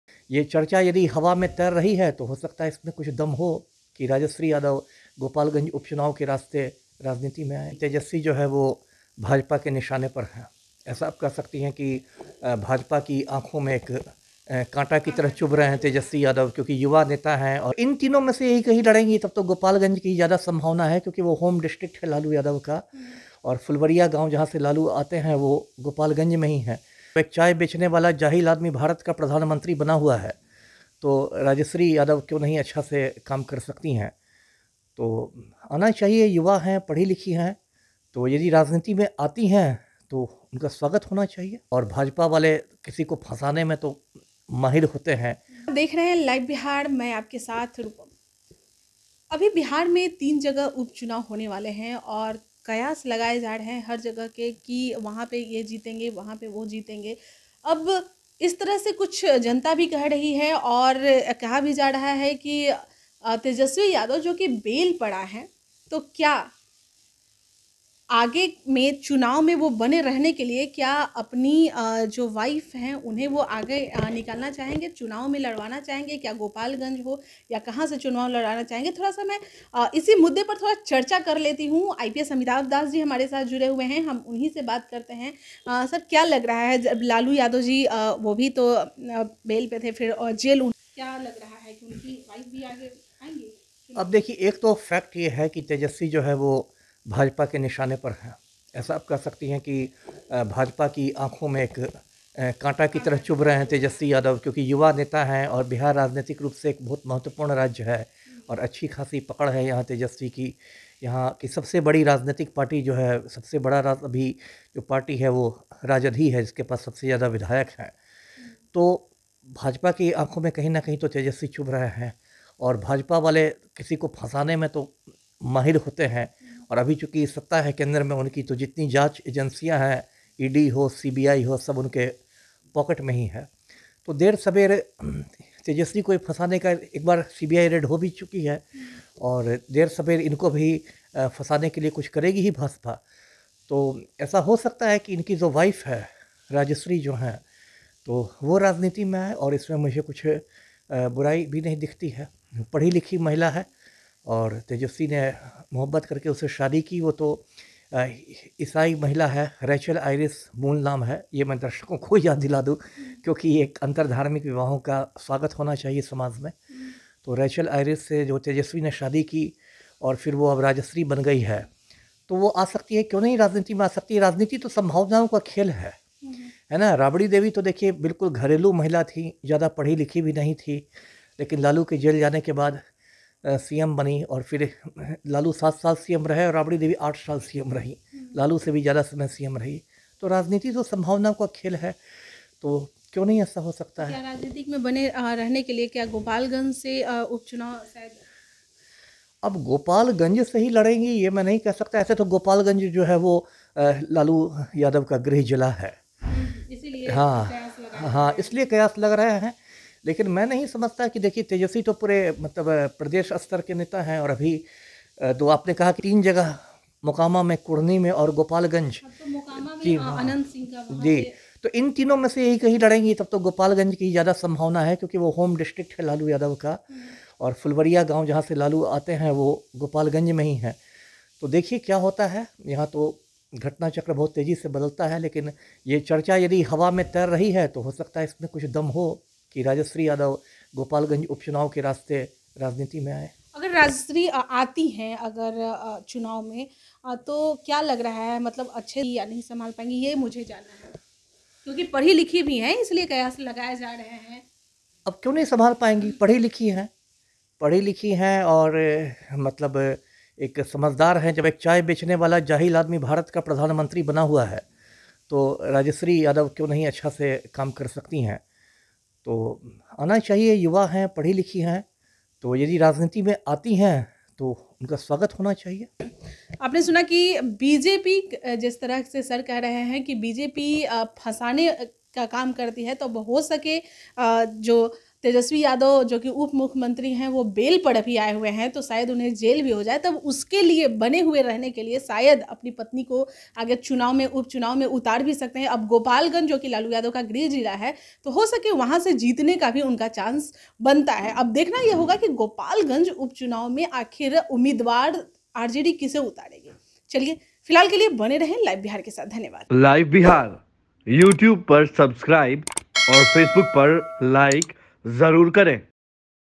गोपालगंज में पत्नी राजश्री को उतारेंतेजस्वी, संकट बड़ा है, तैयारी जरुरी पूर्व IPS अमिताभ दास
Live Bihar Digital Network